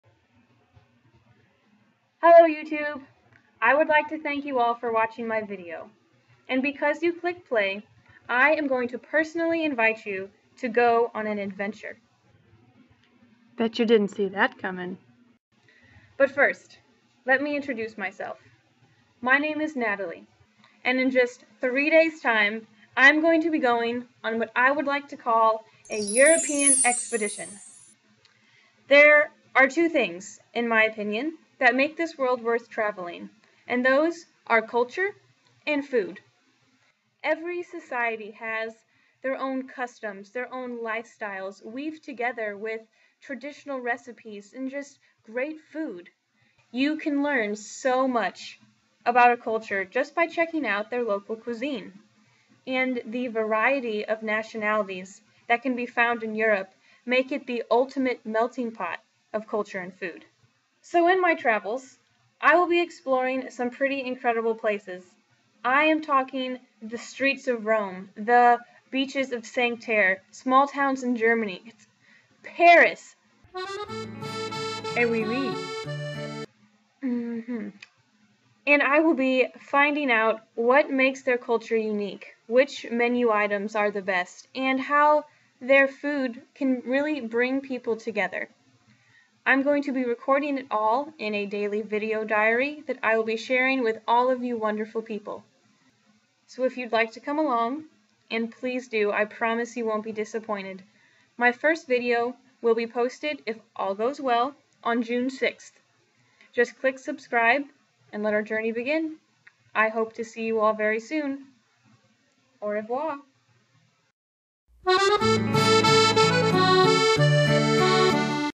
Culinary Travels: You're Invited!
Come follow me as i make my way across Europe exploring the many cultures and wonderful food.